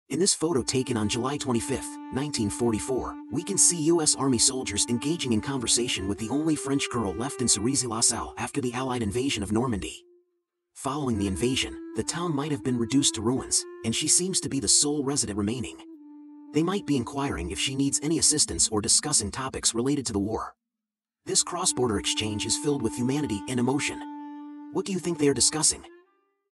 U S Soldiers and the Lone French Girl in Cerisy la Salle, 1944#NormandyInvasion
In this photo taken on July 25, 1944, we can see U.S. Army soldiers engaging in conversation with the only French girl left in Cerisy-la-Salle after the Allied invasion of Normandy. Following the invasion, the town might have been reduced to ruins, and she seems to be the sole resident remaining. They might be inquiring if she needs any assistance or discussing topics related to the war. This cross-border exchange is filled with humanity and emotion. What do you think they are discussing?